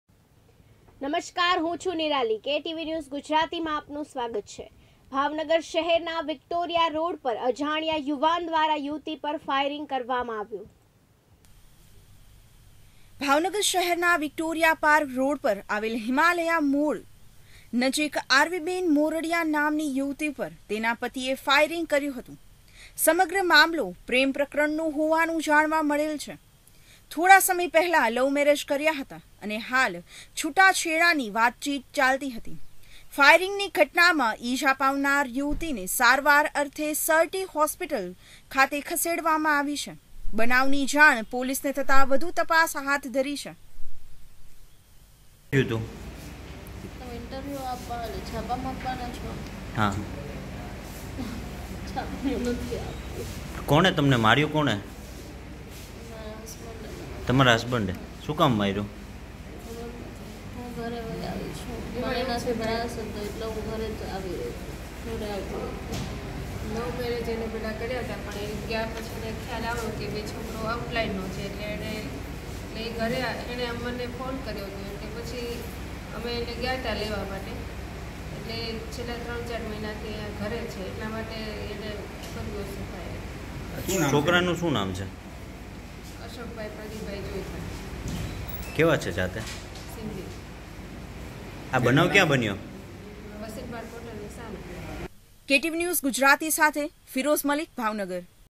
ભાવનગર શહેરના વિકટોરીયા રોડ પર અજાણ્યા યુવાન દ્વારા યુવતી પર ફાયરિંગ કરવામાં
ભાવનગર શહેરના વિકટોરીયા રોડ પર અજાણ્યા યુવાન દ્વારા યુવતી પર ફાયરિંગ કરવામાં આવ્યું